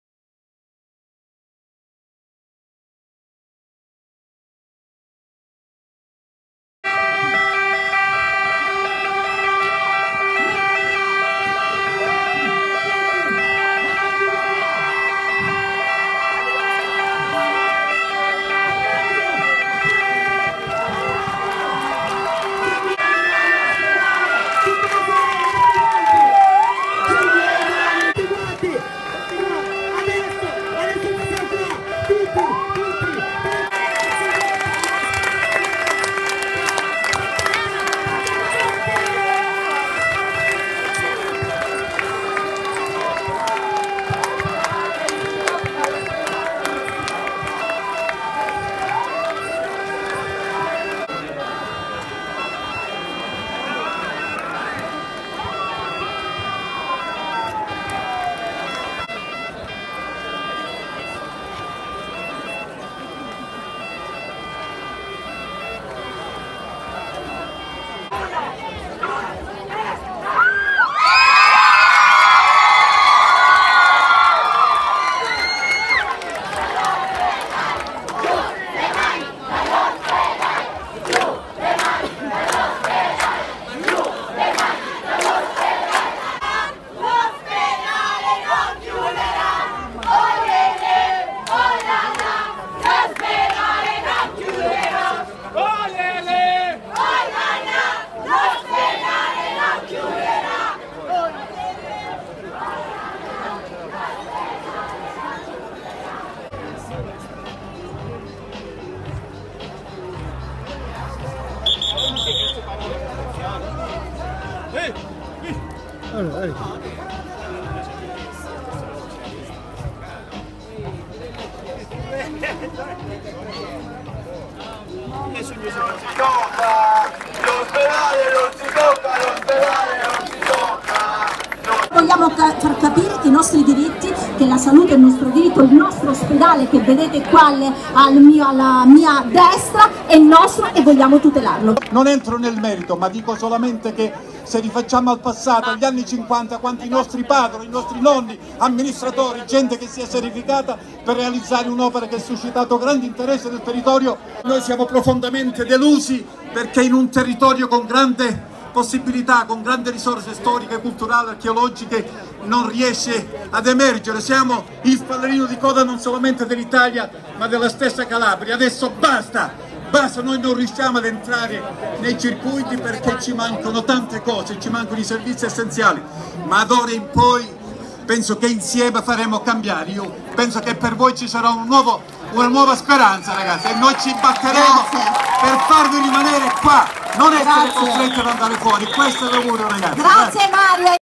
Manifestazione contro chiusura ospedale Locri
Una grande manifestazione di cittadini e studenti della Locride e della Piana ha attraversato il paese per confluire all'Ospedale destinato a essere declassato e quindi a perdere alcune sue funzionalità. Questa protesta si è inserita in una situazione di forte degrado sociale per la crisi  e di abbandono  e per la mancanza di prospettive in questa zona.